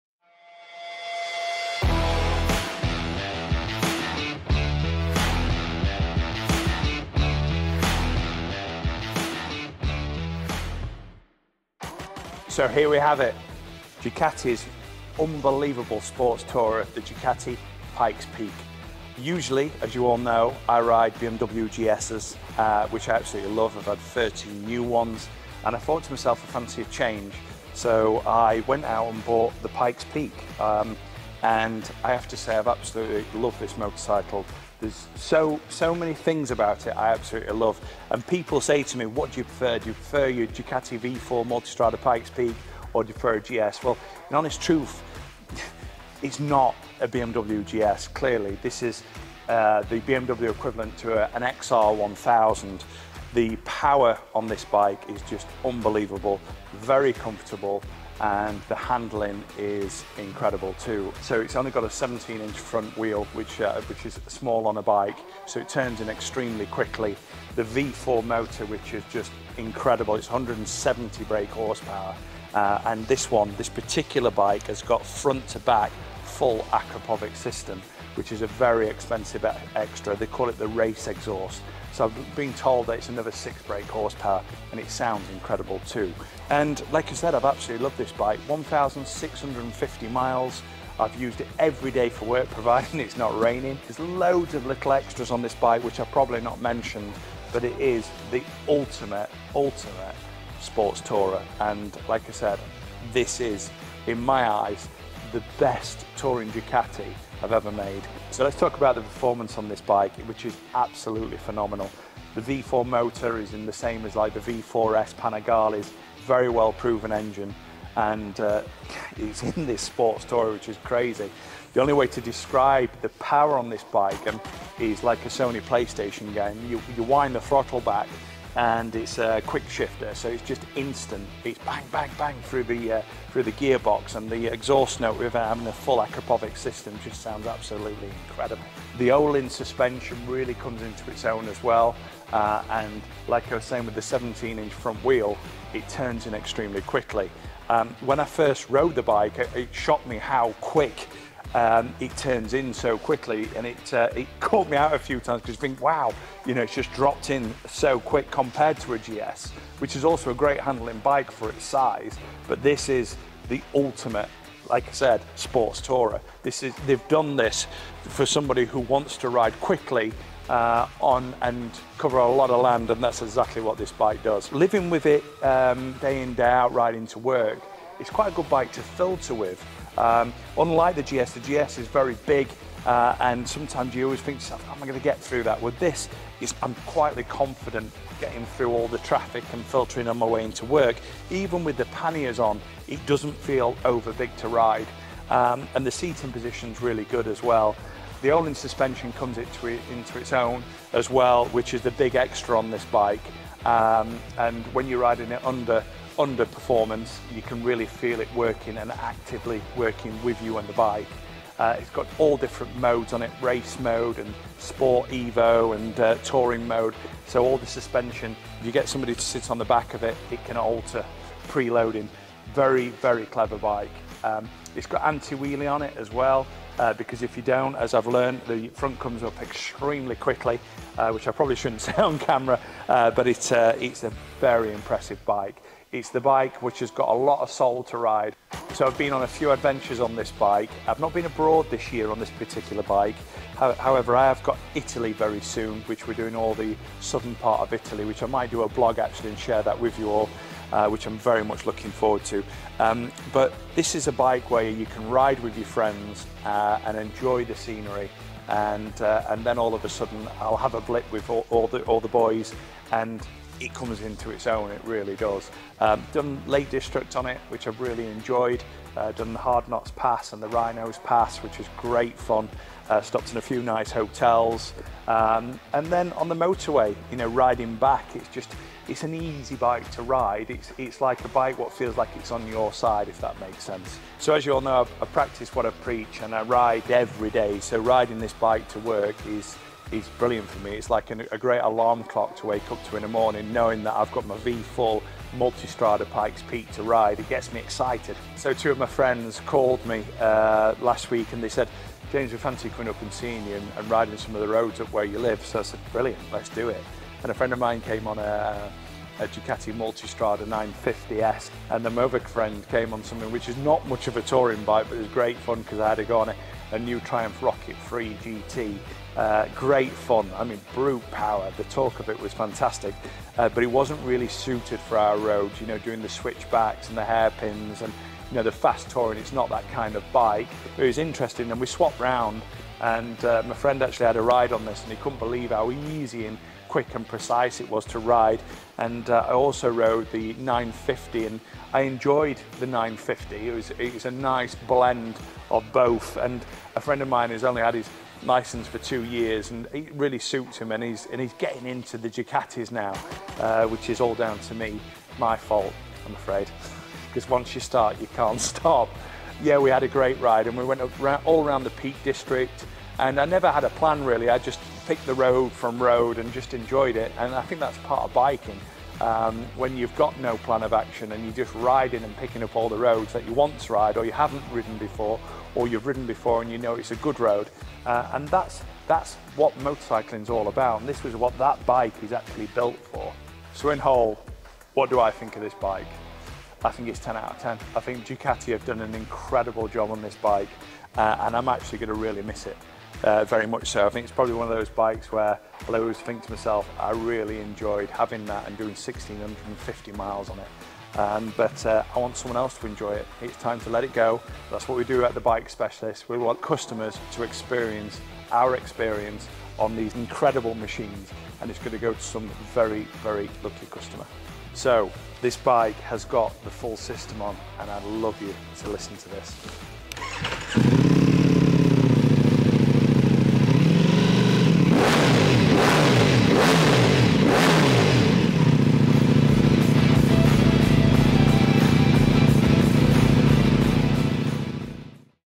Staff Rides: James Holland
The first episode in a new series from TheBikeSpecialists_uk !

Staff Rides! Episode 1 featuring no other than the boss himself, James Holland and his Ducati Multistrada Pikes Peak!